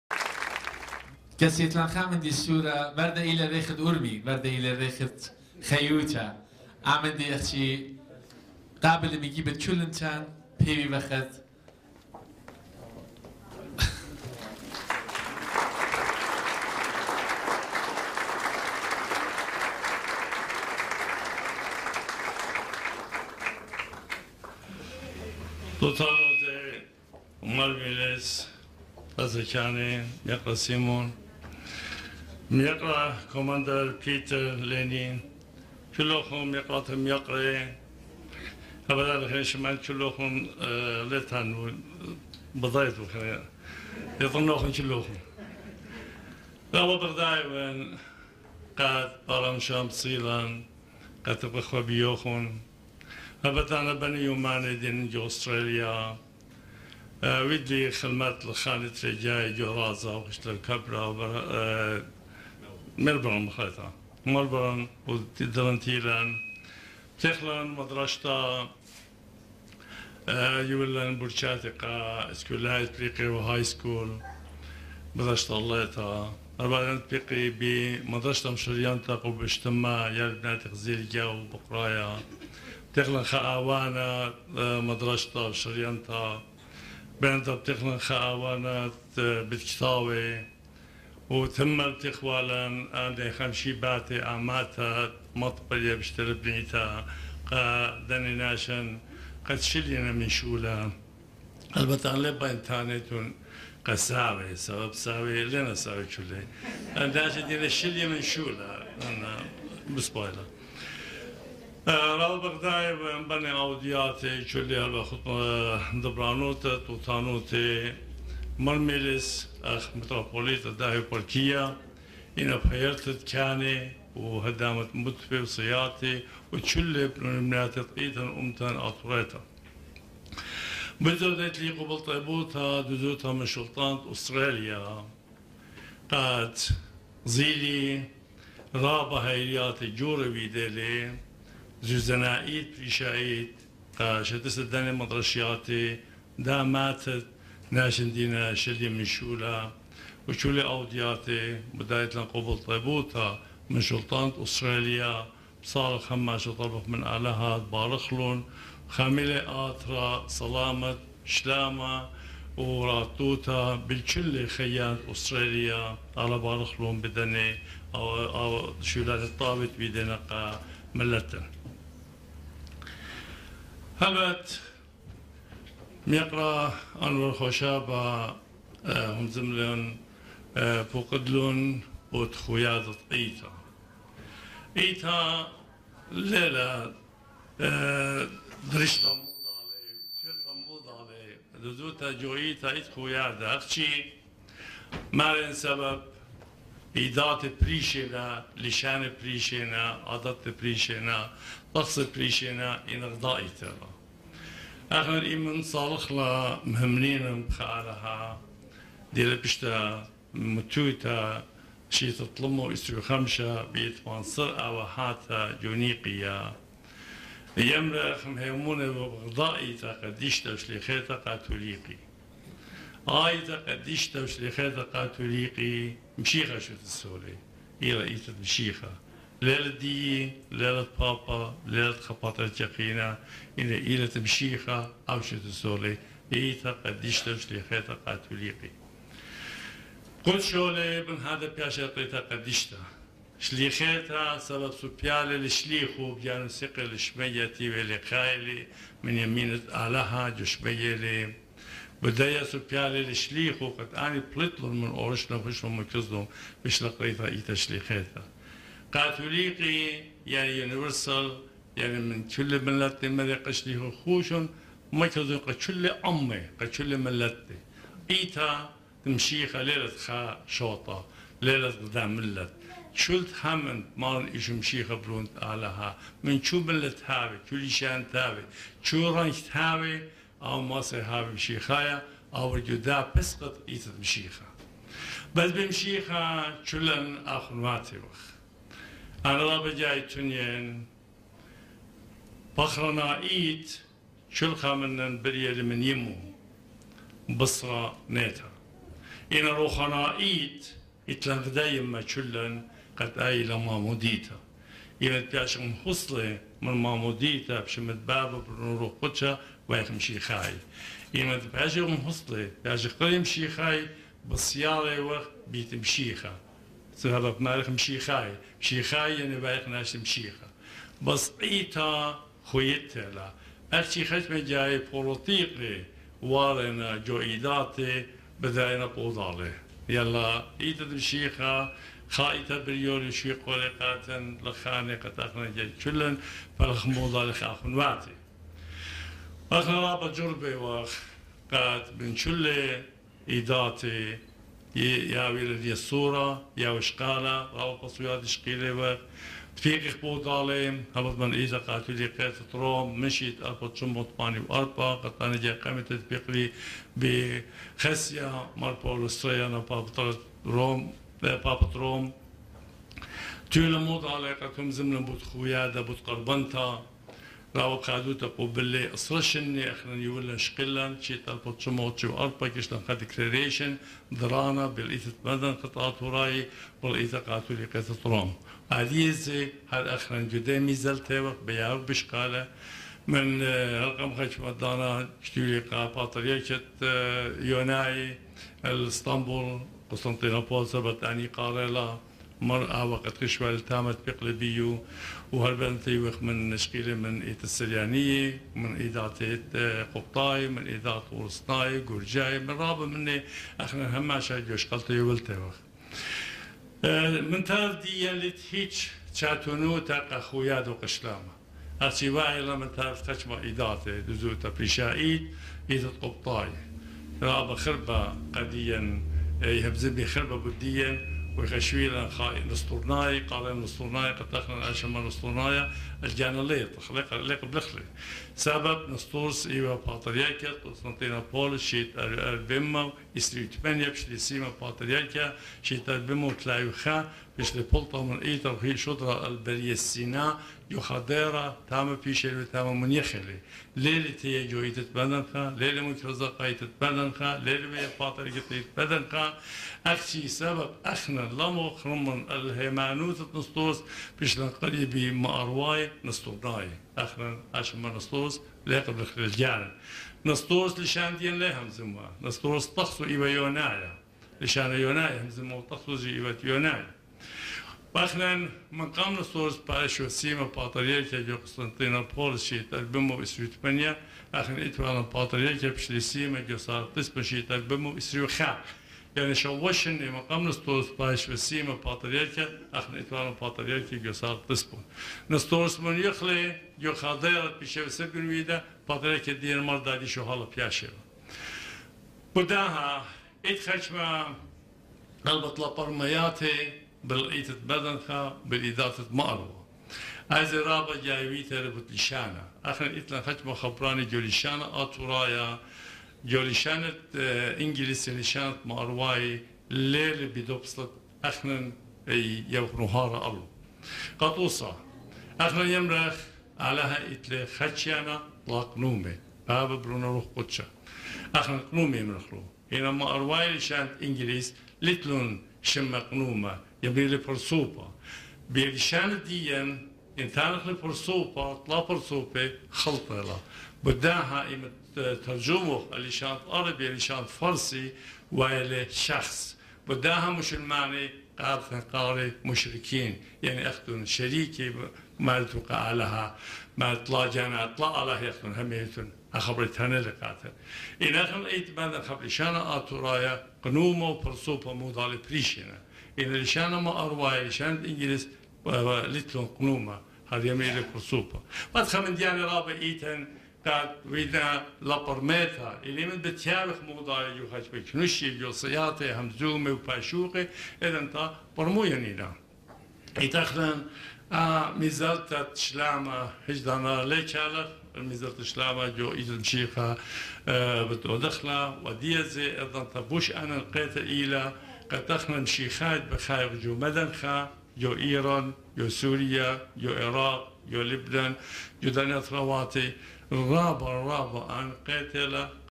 سخنرانی مرحوم پاتریارک ماردنخای فقید، رهبر جهانی کلیسای شرق آشوری در سفر به استرالیا - بخش اول
Lecture by the late Patriarch Mardankha, world leader of the Assyrian Church of the East on a trip to Australia
سخنرانی مرحوم پاتریارک ماردنخای فقید، رهبر جهانی کلیسای شرق آشوری در سفر به استرالیا